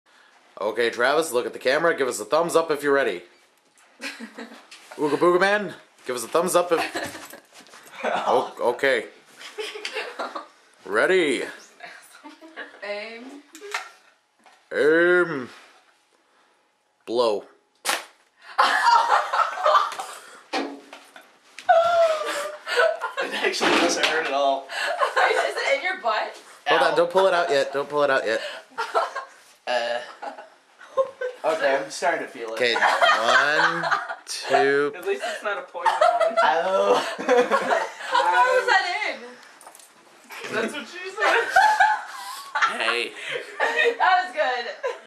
Blow Dart Gun to the Butt
I got shot in the Butt by a blow dart gun, and its awesome